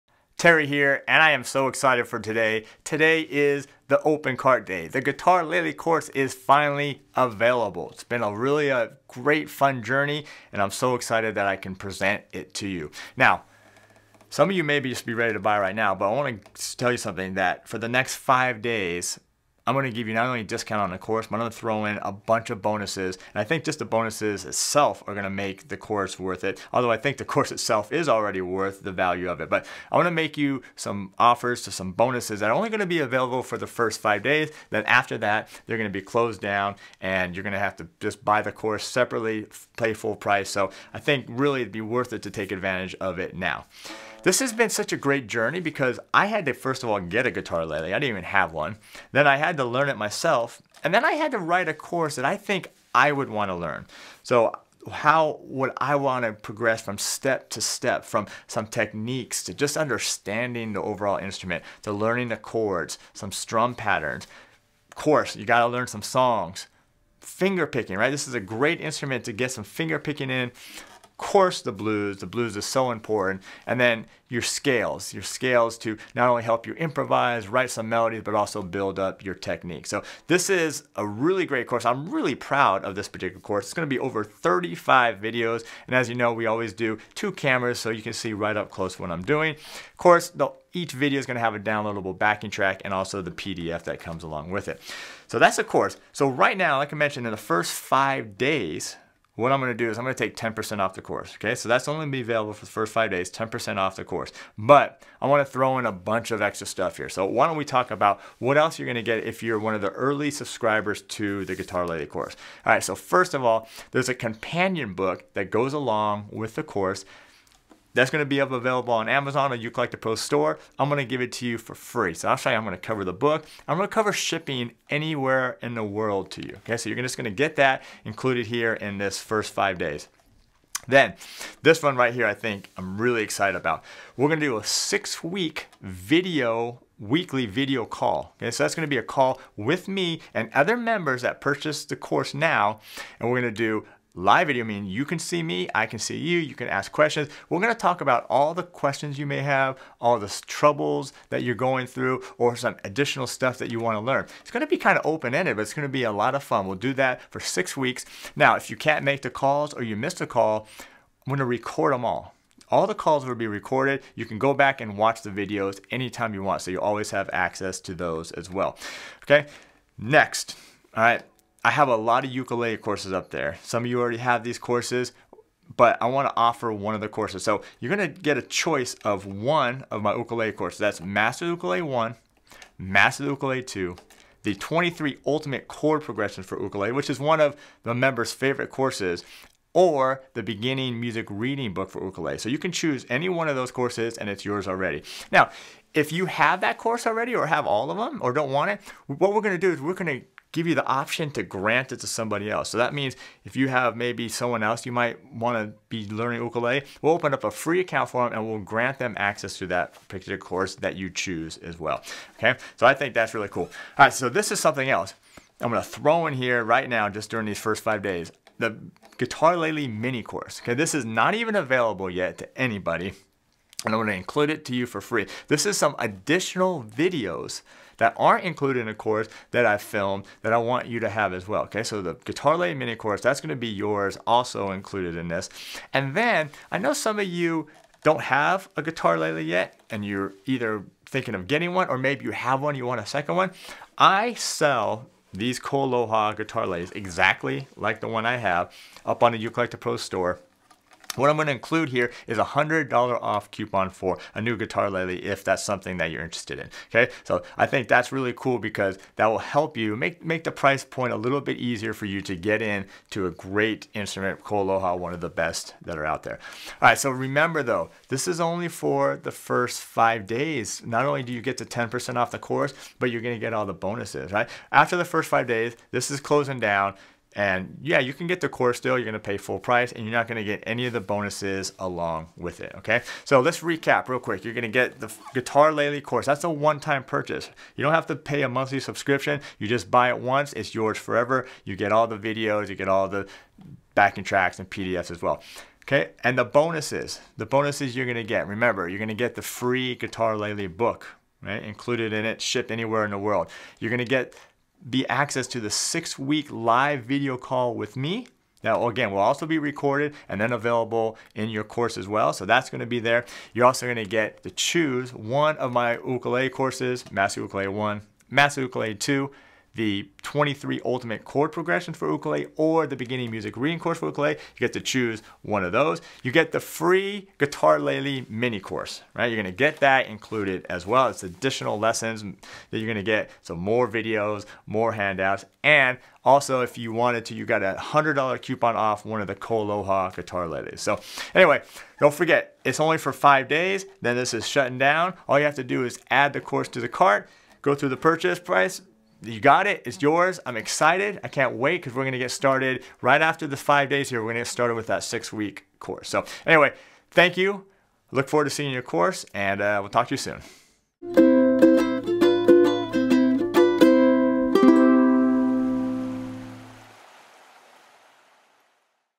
Guitarlele Course For Ukulele & Guitar Players now LIVE
What's in the Guitarlele Course?

This is a first of it's kind complete course for guitarlele.

As you may have noticed there is not a lot of material (videos or books) out there for guitarlele.

Most of the guitarlele videos on YouTube are classical guitar players playing standard repertoire classical pieces.

Although they are great to watch, there is little educational value in them.

If your like me I have been seeing guitarlele everywhere on social media.

It seems like every ukulele and guitar manufacture out there is now making a guitarlele (KoAloha, Yamaha, Flight, Gretsch, Kanilea, Kala). This is totally cool as the instrument becomes more and more popular but how are you going to learn without proper instruction.

Well you have come to the right place.

I have taken my 25 years of teaching experience and designed a personalized guitarlele course that is perfect for the beginning to intermediate guitarlele player.

Not only did I get valuable feedback from the Uke Like The Pros Community in designed this course but I also asked myself, "What would I want to learn after taking this course?"

I came up with a course that is designed to walk you through a step-by-step pathway to ensure you learn all the necessary techniques & styles to make you a great guitarlele player.

* Strumming Fundamentals
* Essential Songs
* Fingerstyle
* Advanced Songs
* The Blues
* Scales

What exactly will you get with the course?

* Over 35 instructional guitarlele videos
* Killer slow and fast play-a-long tracks.
* Easy to read sheet music and TAB
* Confidence to know that you are learning the proper way to becoming a great musician.
* 100% money back guarantee if your not satisfied with the course.

LIMITED TIME BONUSES

Only Available until Friday November 23rd at 11:59 PM PST

* Free Guitarlele companion book ($30 value)
* 6 week LIVE video call with Terry ($900 value)
* Free ukulele course of your choice ($97 Value)
* Free Guitarlele Mini-Course ($47 Value)
* $100 off a KoAloha guitarlele at the Uke Like The Pros store
* 100% Money Back Guarantee

Don't forget the Limited Time Bonuses expire on Friday night November 23rd at 11:59 PM PST.

Terry